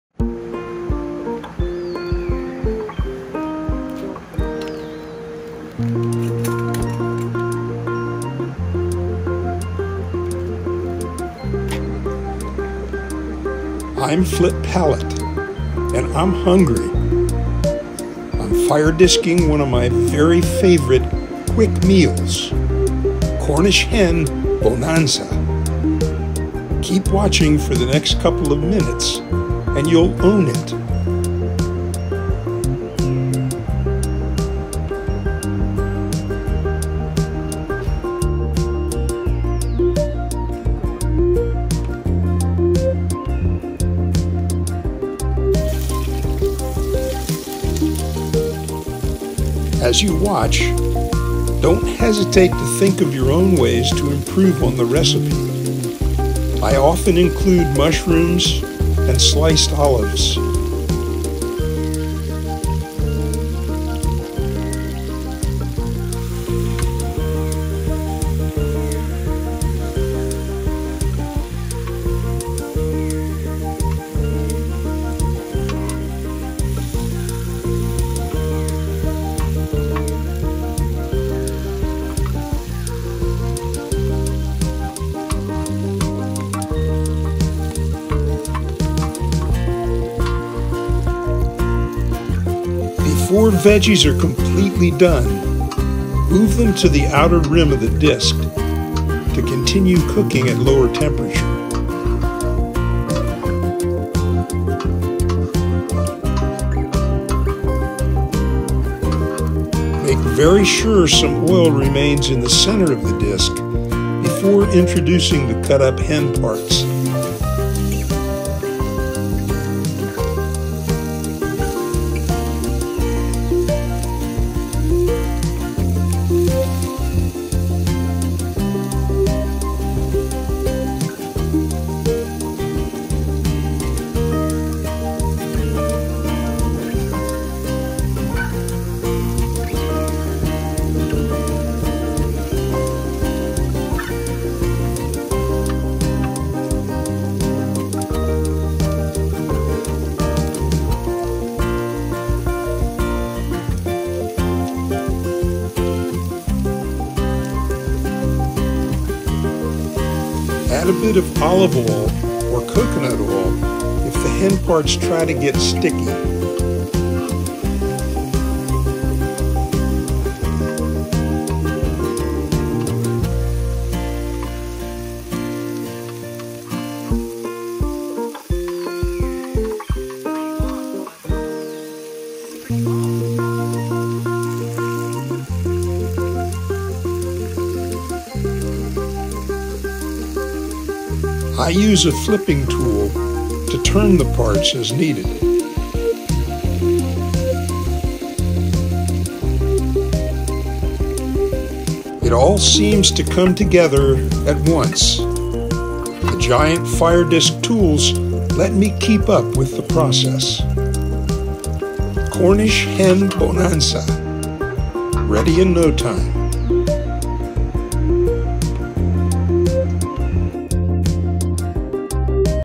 Flip Pallot's Cornish Hen Bonanza on the @firedisccookers Cooker
Flip shares one of this favorite @firedisccookers  recipes. Super easy, quick and tasty meal. Cooking outside is the best!